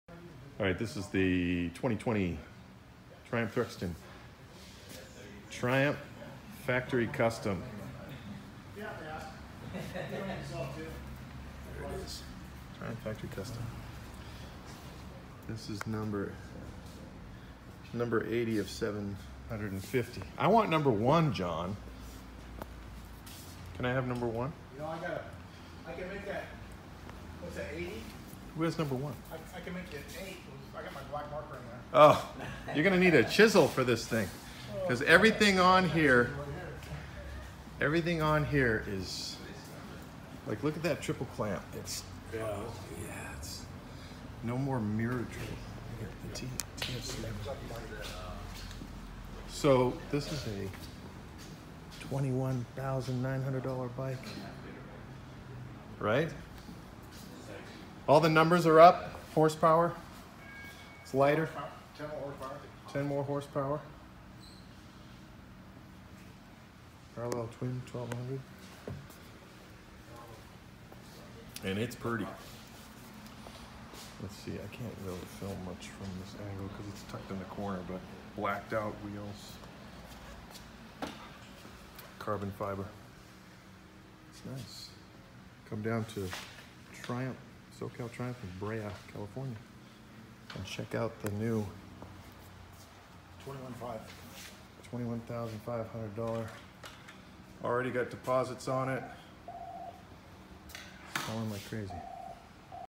Triumph Thruxton TFC Walkaround
A quick look at the 2020 Triumph Thruxton TFC.  Brand new arrival at SoCal Triumph in Brea, California.